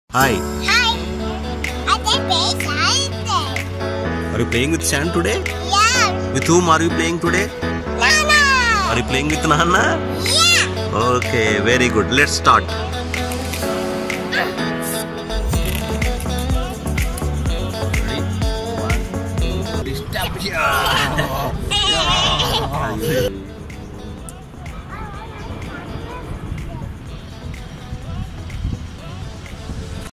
Valentine's Day With Daddy | Playing In The Sand | Celebration
I am Playing in Sand With Nana on Valentine's Day. This is best celebration for me on this Valentine's Day. Love Your Family. There is no boundary for Love..